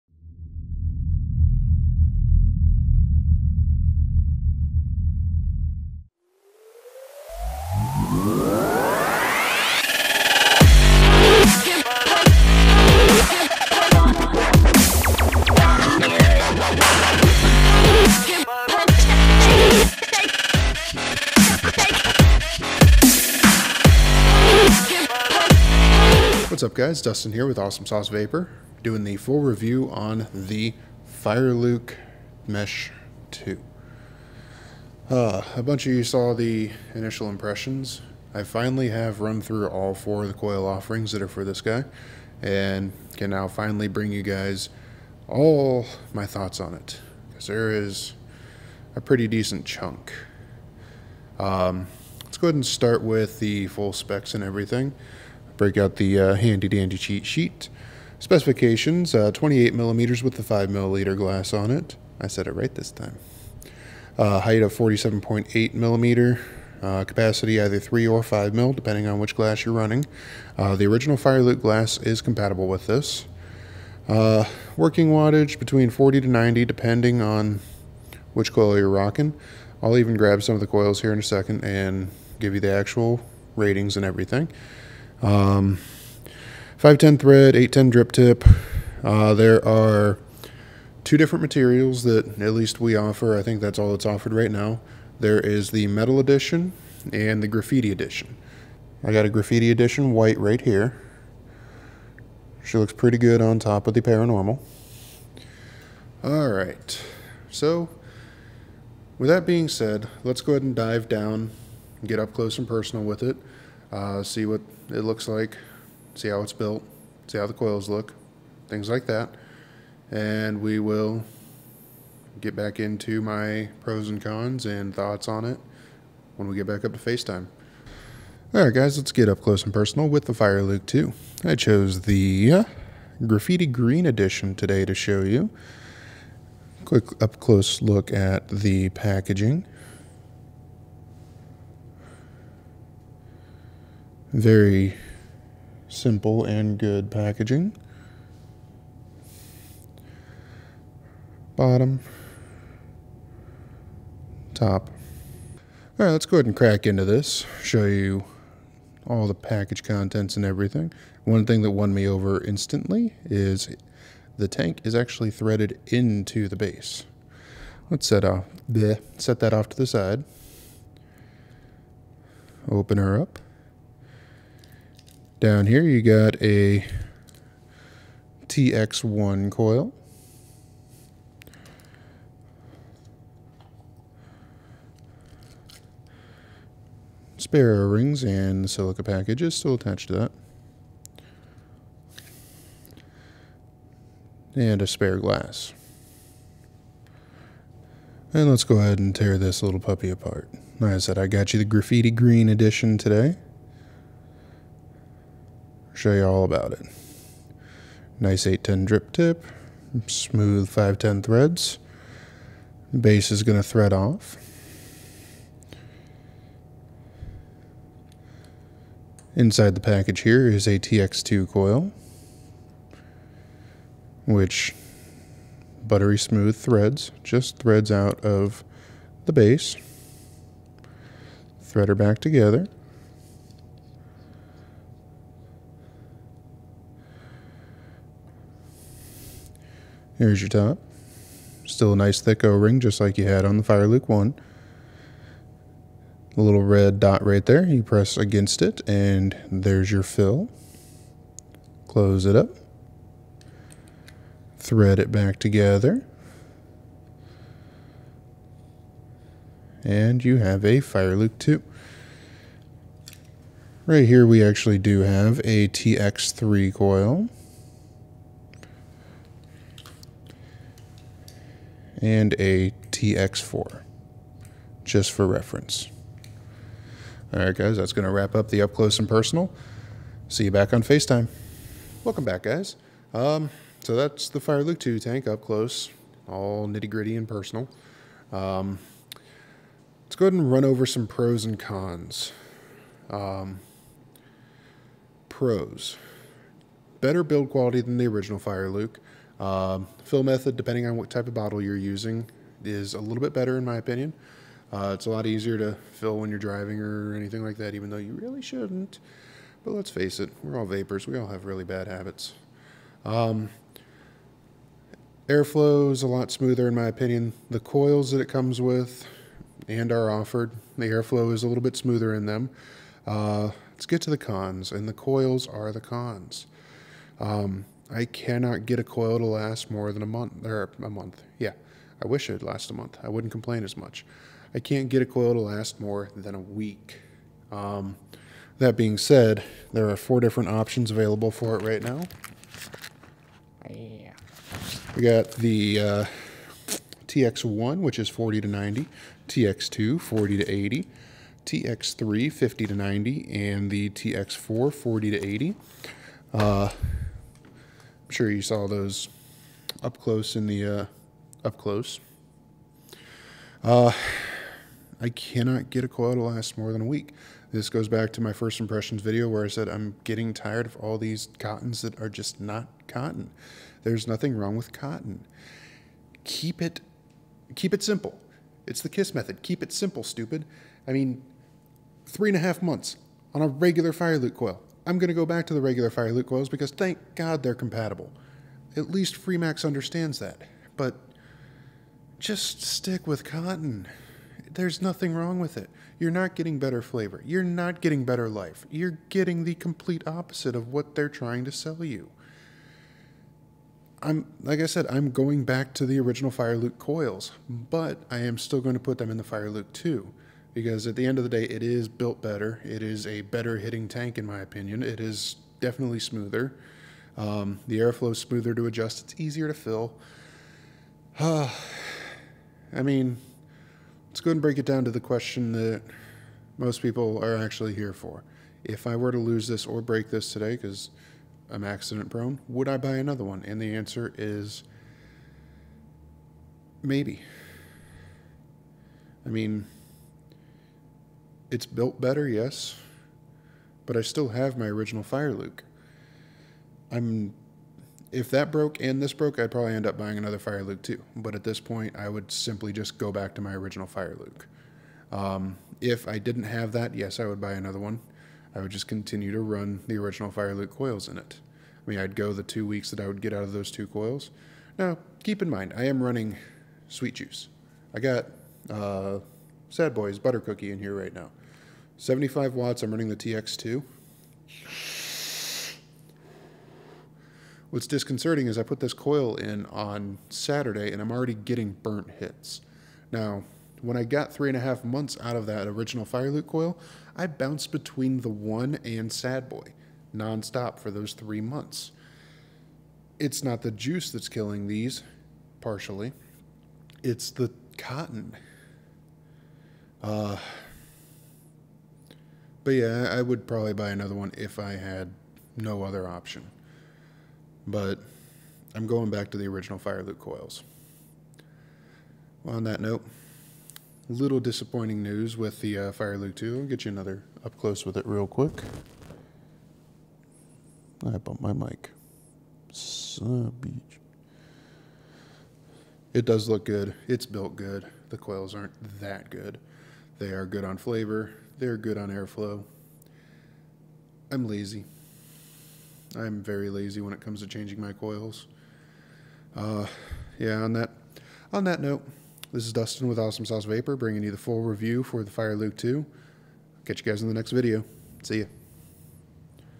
FREEMAX FIRELUKE 2 - FULL REVIEW - Awesome Sauce Vapor Reviews
The FULL review (and rant) for the Freemax Fireluke 2! What's got me so heated on this review? Take a look and let me know what you think!